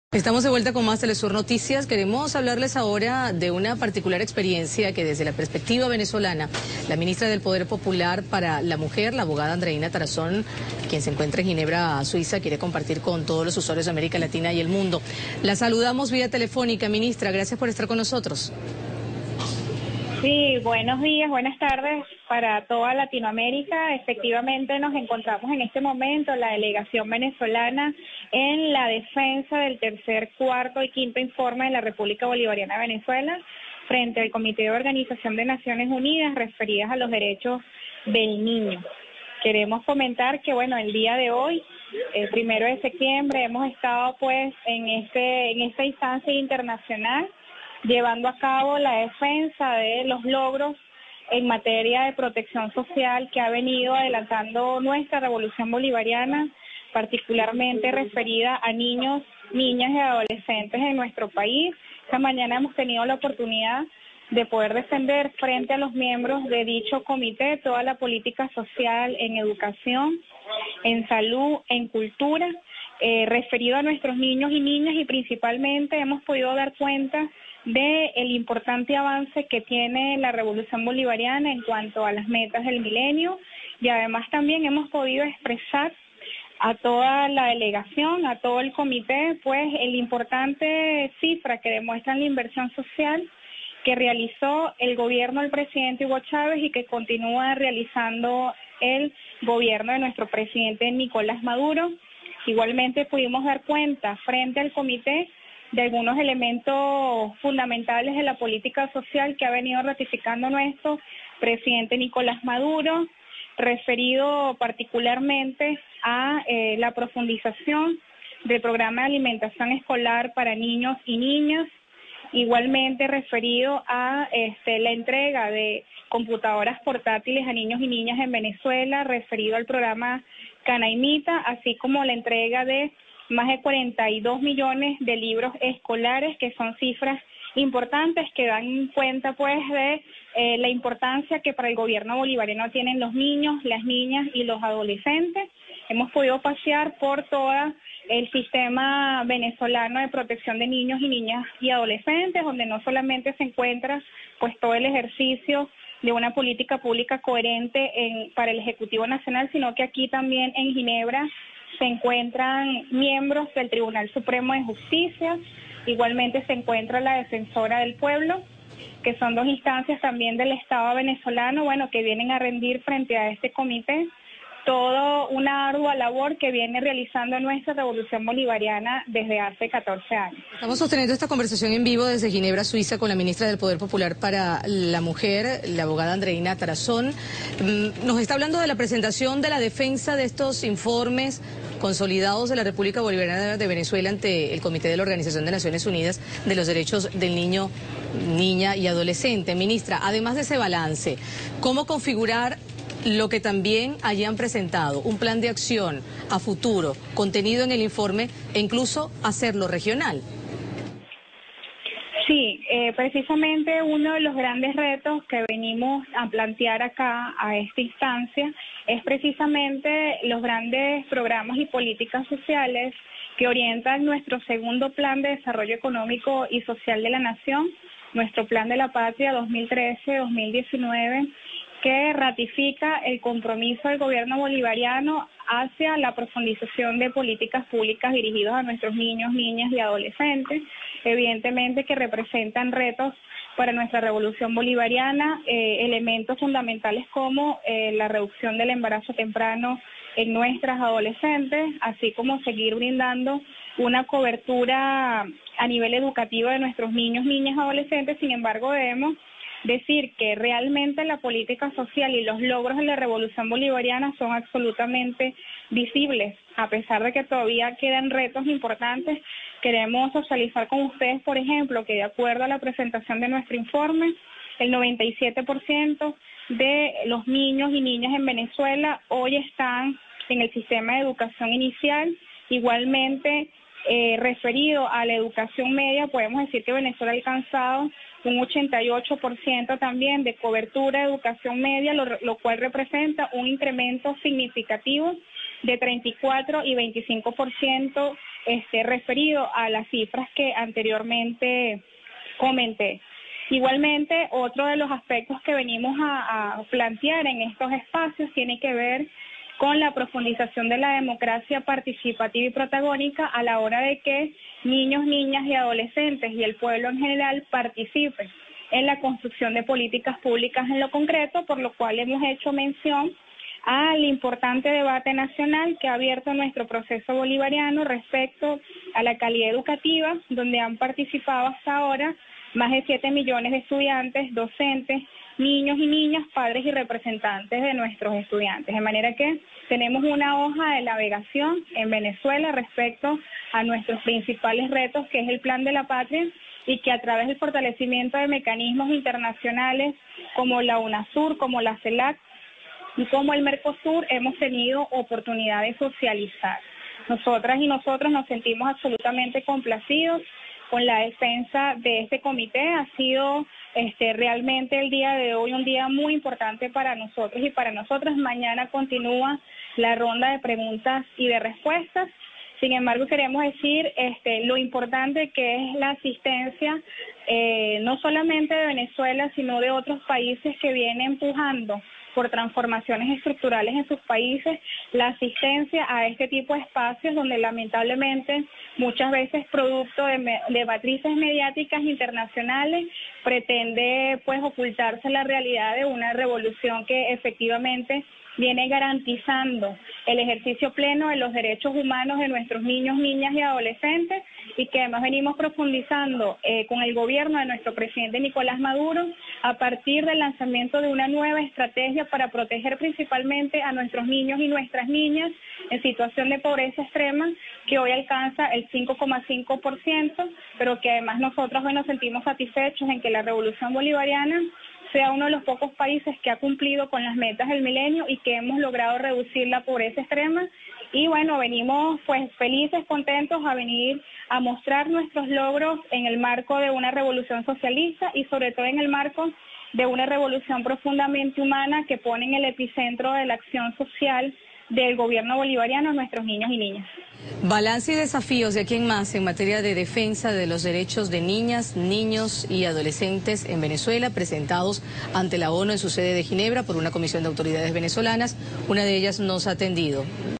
Venezuela presenta informe ante Comité de los Derechos del Niño y la Niña de la ONU
(Prensa MinMujer 01/09/2014) Este primero de septiembre, la ministra del Poder Popular para la Mujer y la Igualdad de Género, Andreína Tarazón, encabezó la presentación y defensa de los Informes consolidados de la República Bolivariana de Venezuela ante el Comité de la Organización de Naciones Unidas (ONU) de los Derechos del Niño y la Niña en Ginebra, Suiza.

Como jefa de la delegación venezolana, la ministra Tarazón señaló que "tuvimos la oportunidad  de defender frente a los miembros del comité (de los Derechos del Niño y la Niña de la ONU) toda la política social en educación, salud y cultura que adelanta Venezuela para nuestros niños y niñas; además hemos podido dar cuenta del importante avance que ha tenido la Revolución en cuanto a las metas del milenio”, afirmó.

Asimismo, destacó que fueron presentadas las cifras de inversión social por parte de la República Bolivariana de Venezuela que realizó el Comandante Supremo de la Revolución Bolivariana, Hugo Chávez y que actualmente continúa el Presidente Nicolás Maduro.

Destacó la profundización del programa de alimentación escolar para niños y niñas, la entrega de computadoras canaimitas y la entrega de más de 42 millones de libros escolares. “Estas son cifras importantes que dan cuenta de la importancia que tienen los niños y las niñas para la Revolución”, dijo.

De igual forma la también presidenta de la Gran Misión Hogares de la Patria, señaló que uno de los grandes retos del país, “es plantear en esta instancia programas y políticas sociales que orientan nuestro segundo Plan de Desarrollo Económico y Social de la Nación que emana de nuestro Plan de la Patria”.
Aseguró que otro de los retos planteados por la Revolución Bolivariana es la reducción del embarazo temprano y seguir avanzando en la cobertura a nivel educativo de los niños, niñas y adolescentes. “Debemos decir que la política social y los logros de la Revolución son absolutamente visibles a pesar de que todavía quedan retos importantes que queremos totalizar”, agregó.

En materia de educación añadió que “en Venezuela el 97 por ciento de los niños y niñas hoy están incluidos en el sistema de educación inicial, igualmente referido a la educación media, alcanzamos un 88 por ciento de cobertura".

Tarazón se refirió a la creación del Consejo Nacional de Derechos Humanos, en abril de 2014, que recoge las recomendaciones de la Defensoría del Pueblo e incluye la participación de las distintas instituciones del Estado en la defensa de los derechos humanos.

Cabe destacar que representantes del Tribunal Supremo de Justicia y la Defensoría del Pueblo participan en la presentación y defensa de este informe que se extenderá hasta este martes dos de septiembre.

Texto: Máyerlin Márquez/ Fotos: MinMujer